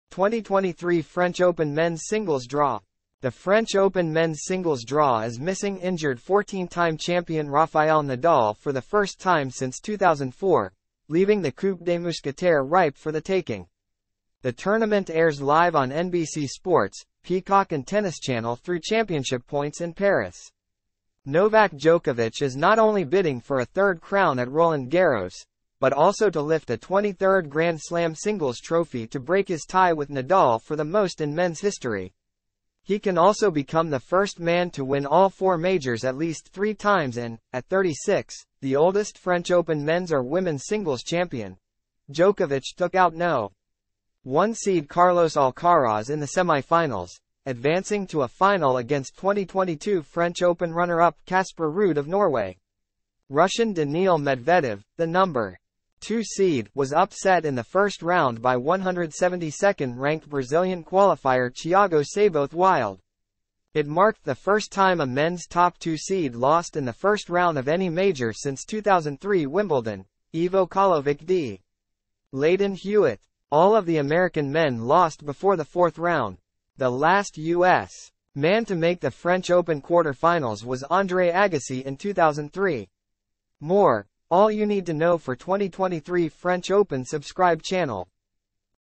2023 French Open men’s singles draw.
The French Open men’s singles draw is missing injured 14-time champion Rafael Nadal for the first time since 2004, leaving the Coupe des Mousquetaires ripe for the taking.
The tournament airs live on NBC Sports, Peacock and Tennis Channel through championship points in Paris.
Novak Djokovic is not only bidding for a third crown at Roland Garros, but also to lift a 23rd Grand Slam singles trophy to break his tie with Nadal for the most in men’s history. He can also become the first man to win all four majors at least three times and, at 36, the oldest French Open men’s or women’s singles champion.Djokovic took out No. 1 seed Carlos Alcaraz in the semifinals, advancing to a final against 2022 French Open runner-up Casper Ruud of Norway.
Russian Daniil Medvedev, the No. 2 seed, was upset in the first round by 172nd-ranked Brazilian qualifier Thiago Seyboth Wild. It marked the first time a men’s top-two seed lost in the first round of any major since 2003 Wimbledon (Ivo Karlovic d. Lleyton Hewitt).
All of the American men lost before the fourth round. The last U.S. man to make the French Open quarterfinals was Andre Agassi in 2003.